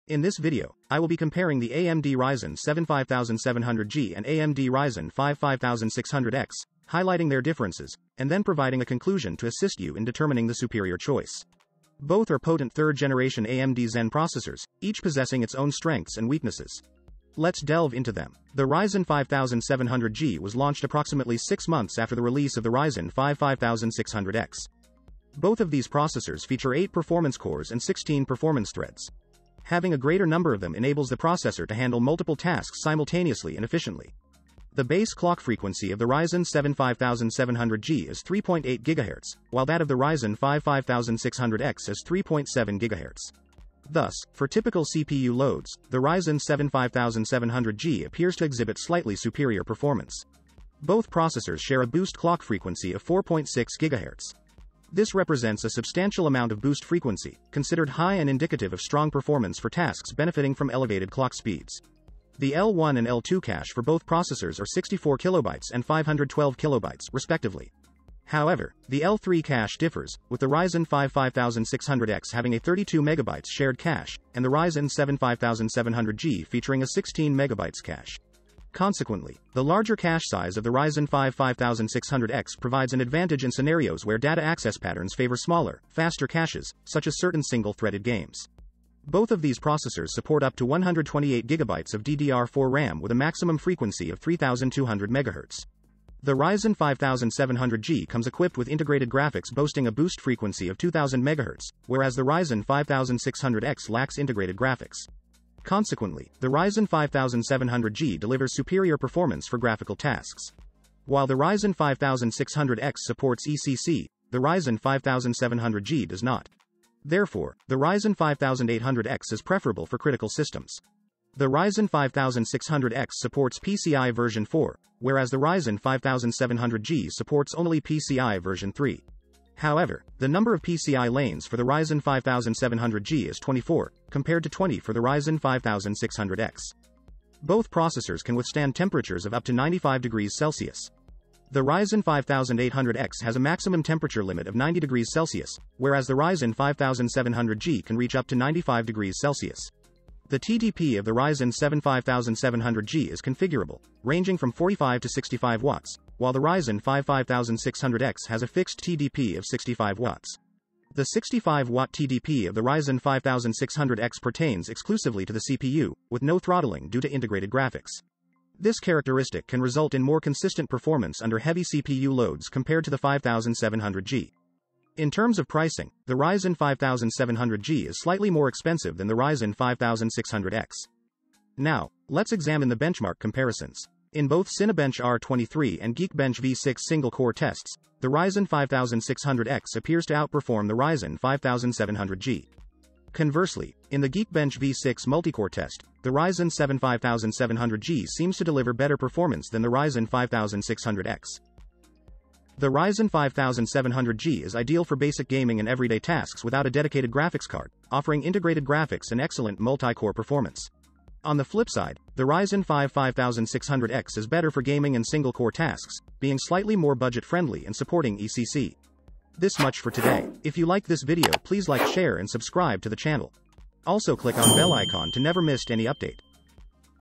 AMD Ryzen 7 5700G vs Ryzen 5 5600X: Which CPU Should YOU Buy?
Explore the showdown between the AMD Ryzen 7 5700G and Ryzen 5 5600X in this in-depth comparison video! Discover the key differences in performance, features, and price to help you make an informed decision for your next PC build or upgrade. Whether you're a gamer seeking top-notch single-core performance or a multi-tasker in need of higher multi-core capabilities, we break down the strengths and weaknesses of each processor. Don't miss out on this comprehensive guide to choosing between the Ryzen 7 5700G and Ryzen 5 5600X – your gateway to a powerful computing experience!